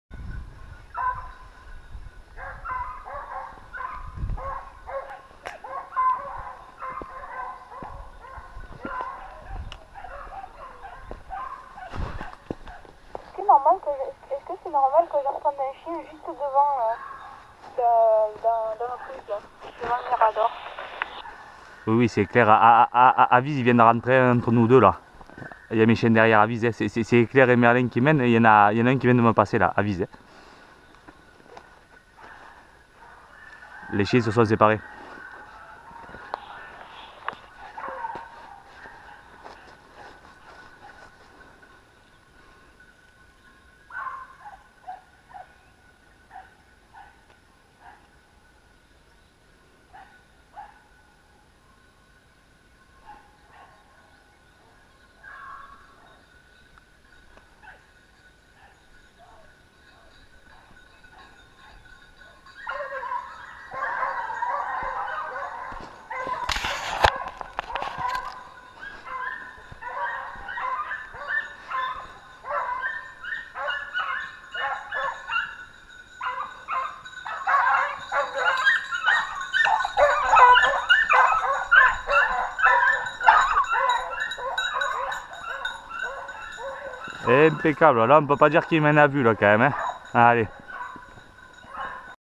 Chasse au sanglier en Cevennes.
Entrainement de petit basset griffon vendéen et basset fauve de Bretagne sur sanglier.On voit le sanglier de 40kg en début de la vidéo et les chiens a la fin de la vidéo.